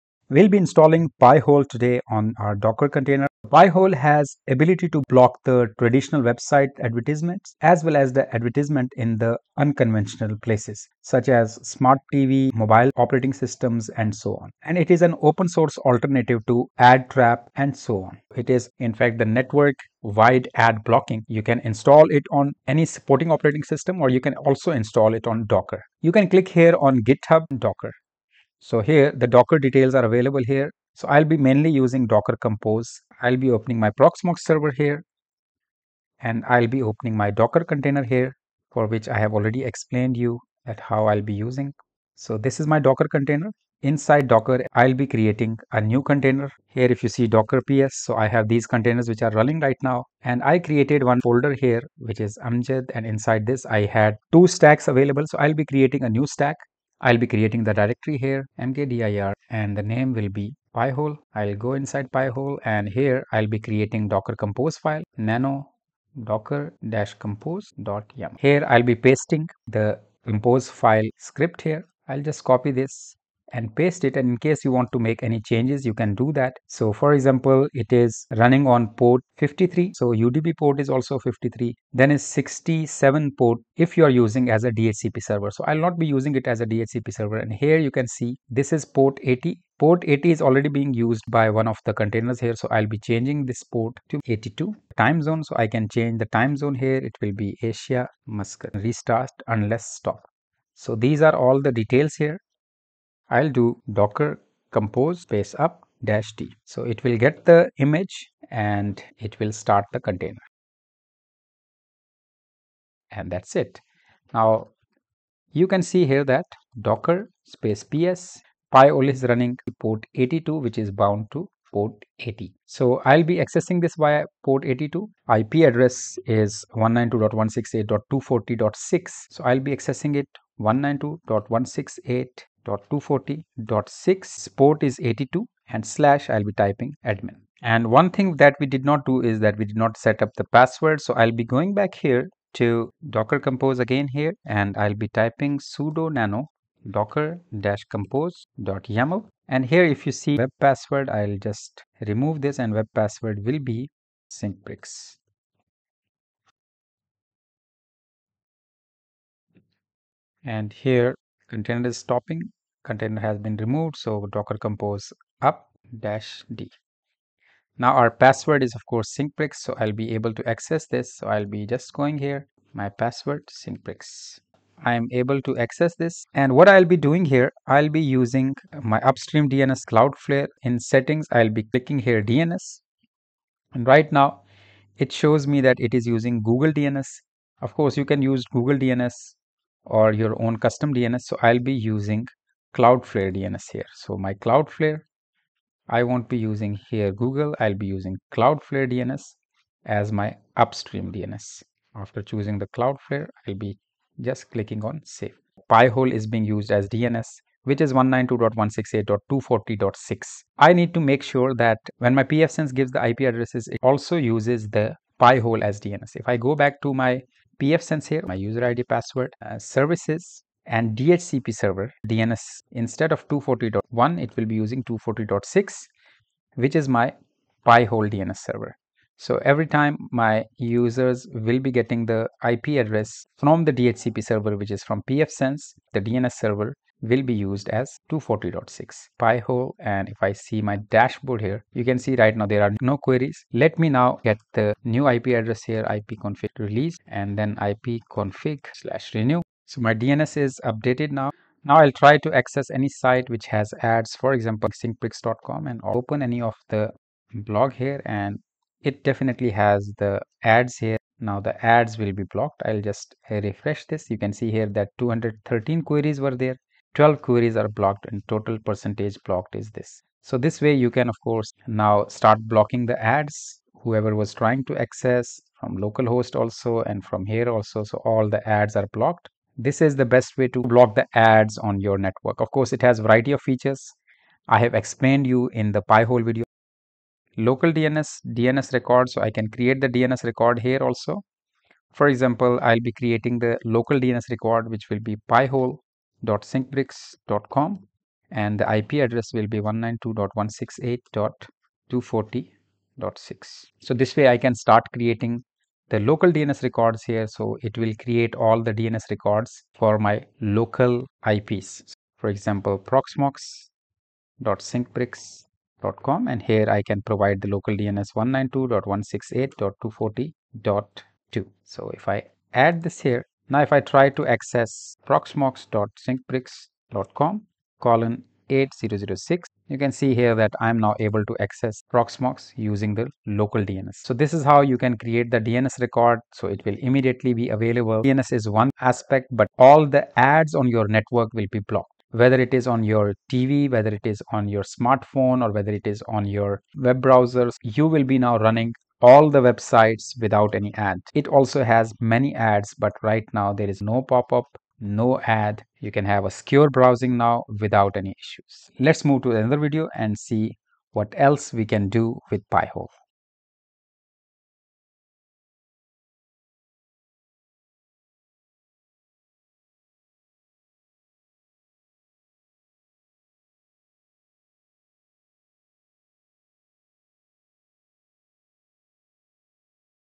Ad Blocking Setup: Pi-hole on Docker with pfSense Integration | Stop Ads Network-wide!
In this step-by-step tutorial, learn how to supercharge your network's ad-blocking capabilities with Pi-hole running on Docker within a Proxmox environment. Discover the seamless integration with pfSense, turning your network into an ad-free zone. From installation to local DNS resolution, we've got you covered!

0:05 - Introduction
0:20 - Installing Pi-hole on Docker (Proxmox)
02:50 -  Pi-Hole Web UI
03:19 - Upstream DNS as Cloudflare
03:37 - Configuring pfSense for Pi-hole DNS
04:20 - Pi-Hole Dashboard
04:48- Local DNS Resolution Setup
06:00 - Conclusion